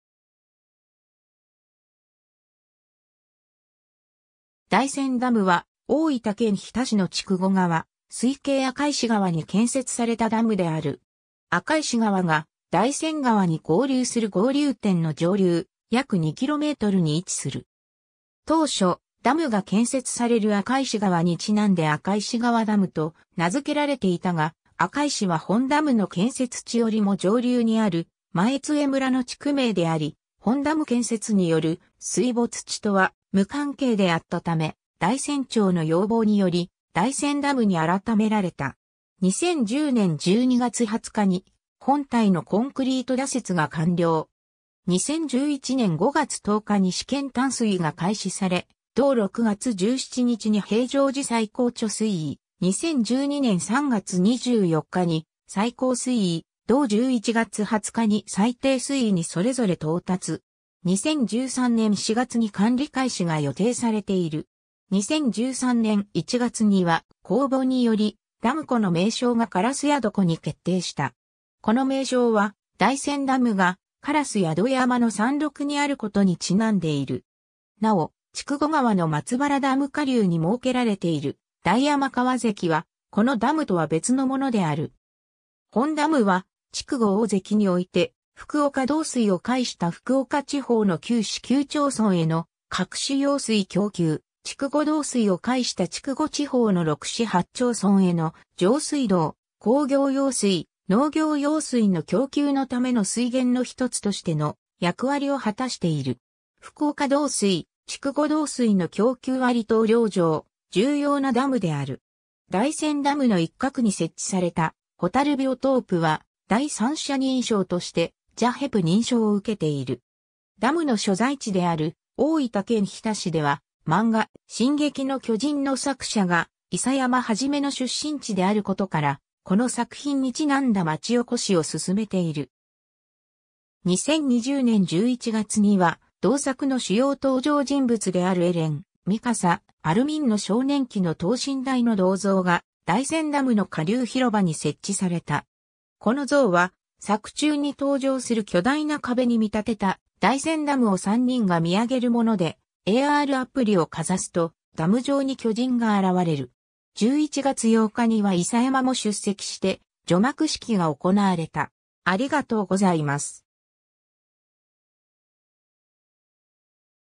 大山ダム
大山ダム（おおやまダム）は、大分県日田市の筑後川水系赤石川に建設されたダムである。
 赤石川が大山川（筑後川の上流部分の別名）に合流する合流点の上流約2kmに位置する。
 当初、ダムが建設される赤石川に因んで赤石川ダムと名付けられていたが、赤石は本ダムの建設地よりも上流にある前津江村の地区名であり、本ダム建設による水没地とは無関係であったため、大山町の要望により大山ダムに改められた。
 2010年（平成22年）12月20日に本体のコンクリート打設が完了。
2011年（平成23年）5月10日に試験湛水が開始され、同6月17日に平常時最高貯水位、2012年（平成24年）3月24日に最高水位（洪水時最高水位）、同11月20日に最低水位にそれぞれ到達。
2013年（平成25年）4月に管理開始が予定されている。
 2013年（平成25年）1月には、公募により、ダム湖の名称が烏宿湖（うしゅくこ）に決定した。
この名称は、大山ダムが烏宿山（うしゅくやま、からとまりやま）の山麓にあることに因んでいる。
 なお、筑後川の松原ダム下流に設けられている大山川堰（別名：大山川ダム）は、このダムとは別のものである。
 本ダムは、筑後大堰において福岡導水を介した福岡地方の9市9町村への各種用水供給、筑後導水を介した筑後地方の6市8町村への上水道・工業用水・農業用水の供給のための水源の一つとしての役割を果たしている。
福岡導水・筑後導水の供給割当量（次表）上、重要なダムである。
 大山ダムの一角に設置された「ホタルビオトープ」は、第三者認証としてJHEP認証を受けている。
 ダムの所在地である大分県日田市では、漫画「進撃の巨人」の作者が諫山創の出身地であることから、この作品に因んだまちおこしを進めている。
2020年11月には、同作の主要登場人物であるエレン、ミカサ、アルミンの少年期の等身大の銅像が、大山ダムの下流広場に設置された。
この像は、作中に登場する巨大な壁に見立てた大山ダムを3人が見上げるもので、ARアプリをかざすとダム上に巨人が現れる。
11月8日には諫山も出席して除幕式が行われた。